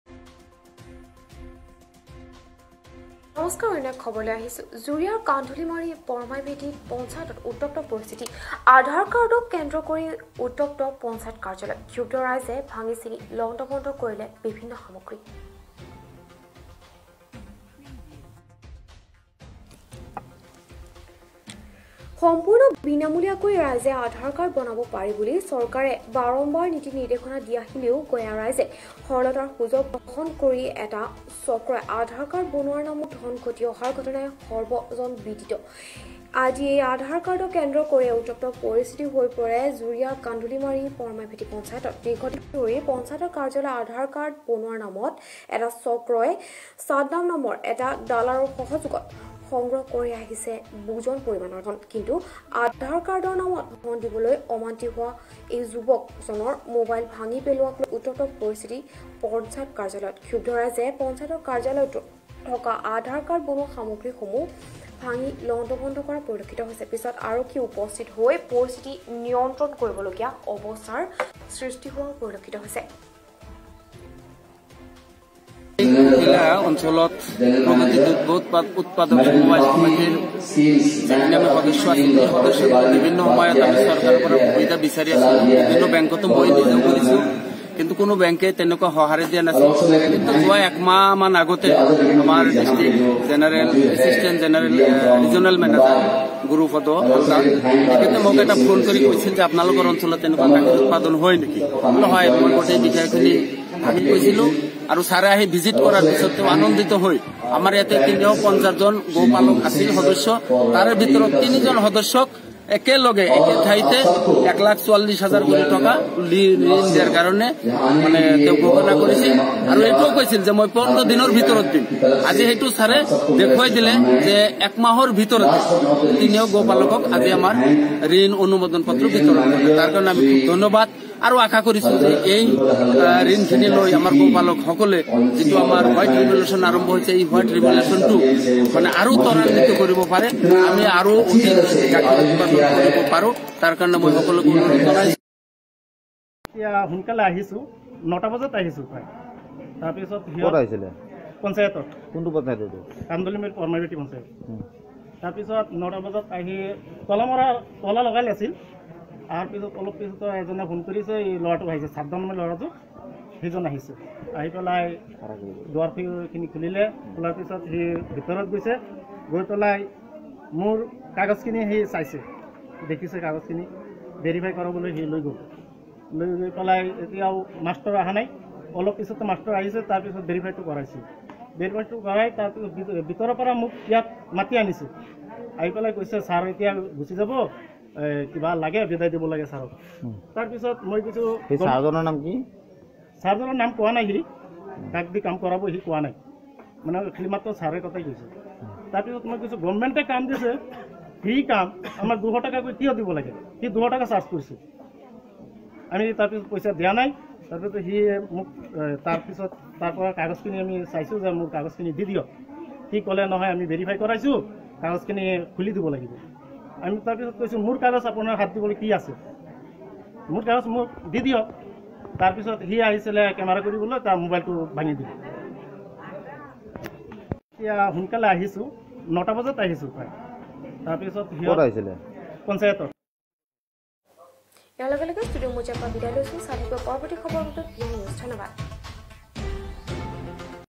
জুৰীয়াৰ কান্ধুলীমাৰী-পৰমাইভেটি পঞ্চায়তত উত্তপ্ত পৰিস্থিতি
আধাৰ কাৰ্ডক কেন্দ্ৰ কৰি উত্তপ্ত পঞ্চায়ত কাৰ্যালয়
ক্ষুব্ধ ৰাইজে ভাঙি লণ্ড-ভণ্ড বিভিন্ন সামগ্ৰী